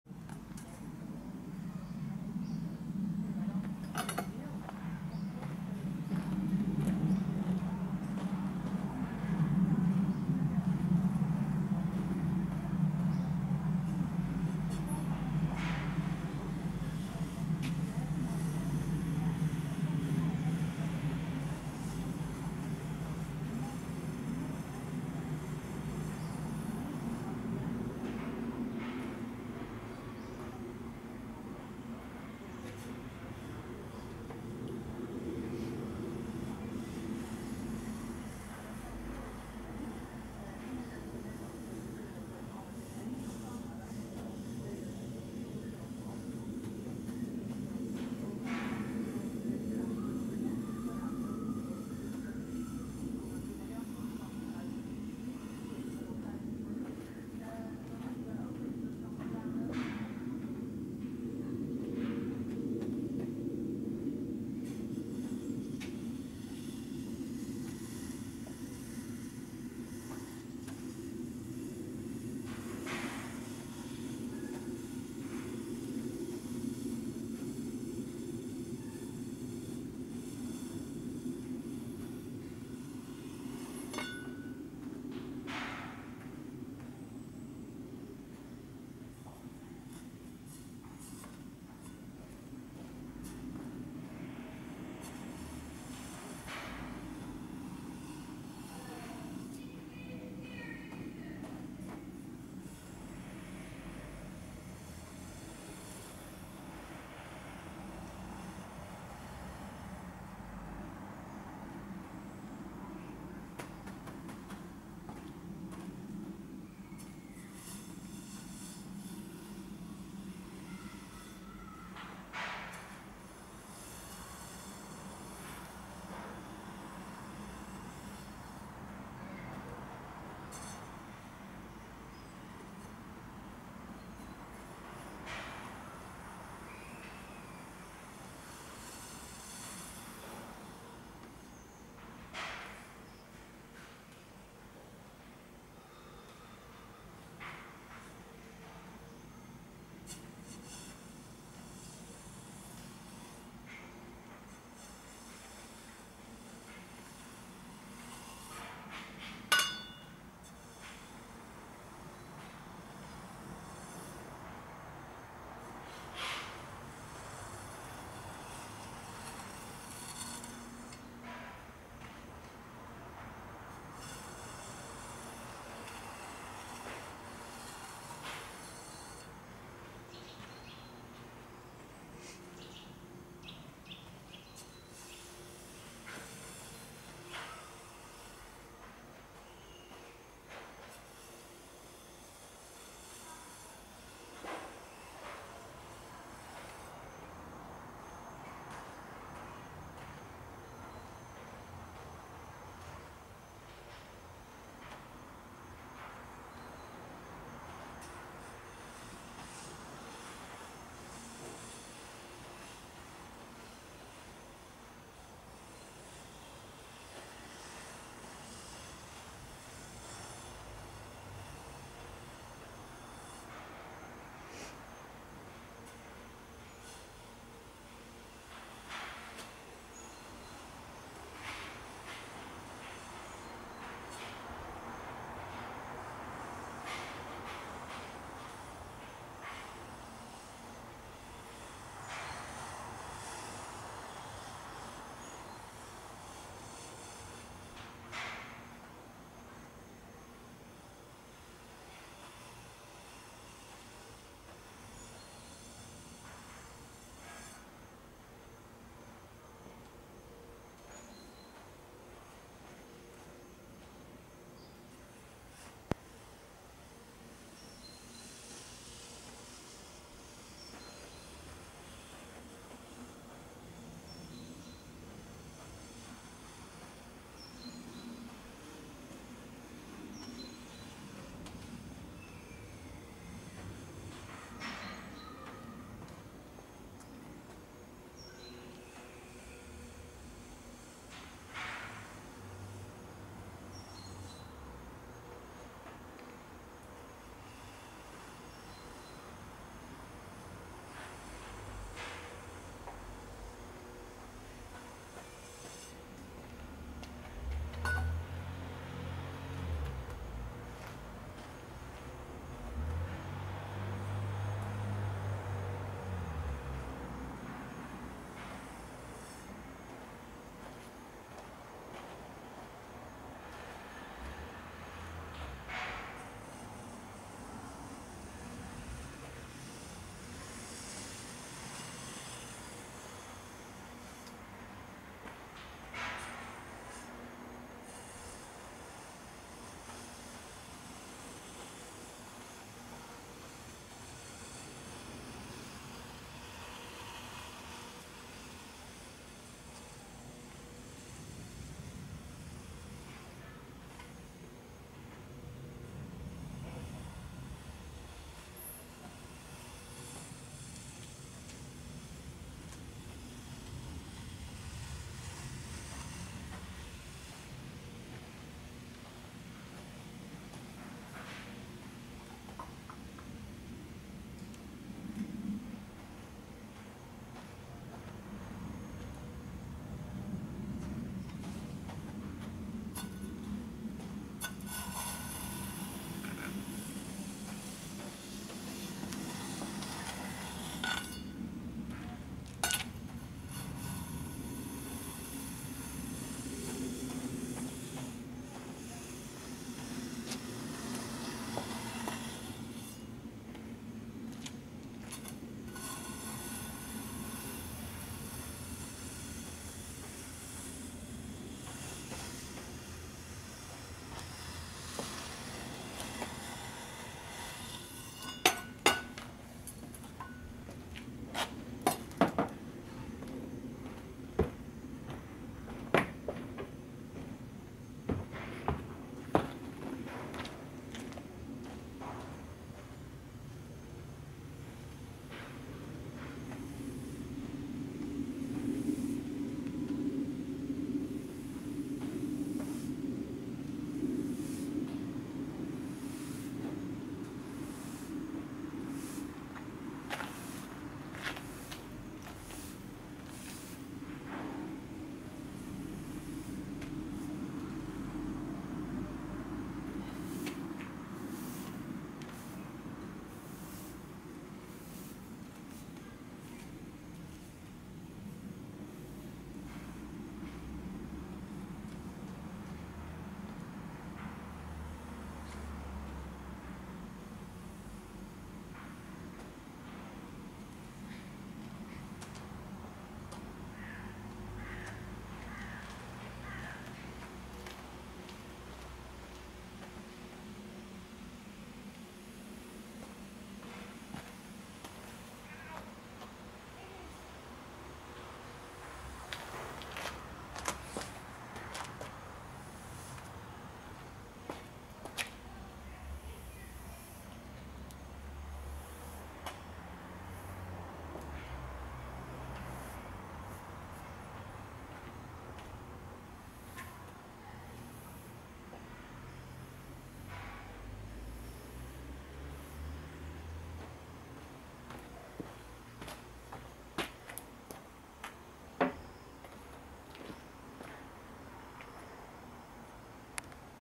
Zen garden raking
Kikiyaya is a private Japanese style garden in the Netherlands.